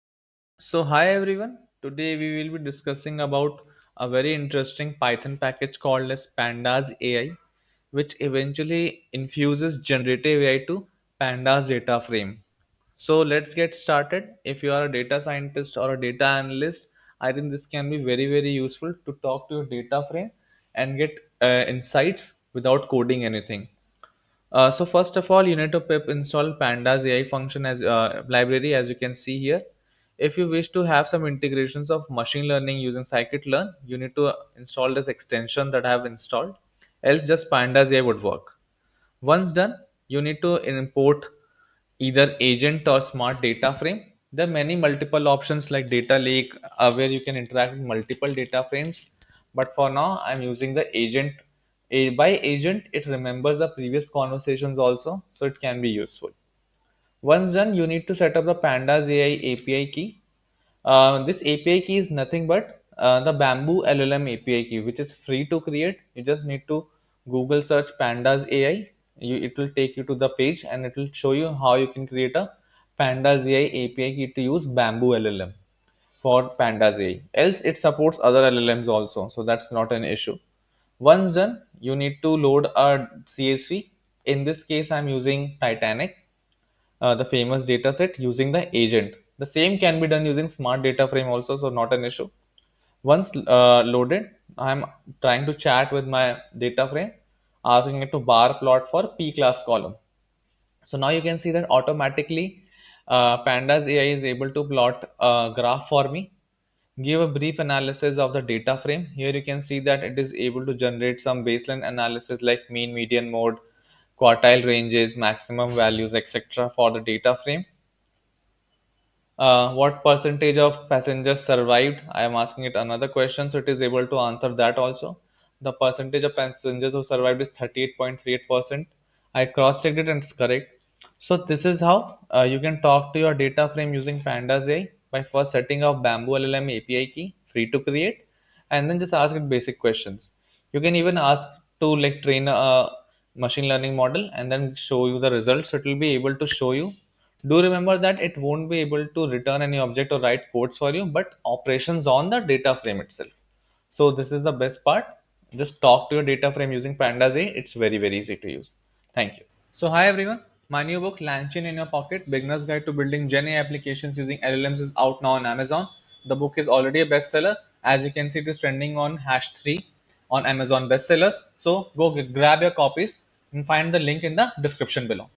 PandasAI : Generative AI for pandas dataframe
This video demonstrates PandasAI, which enables generative AI for pandas dataframe and with this, you can talk to your dataframe object easily.

LangChain in your Pocket Beginners guide to building Generative AI applications using LLMs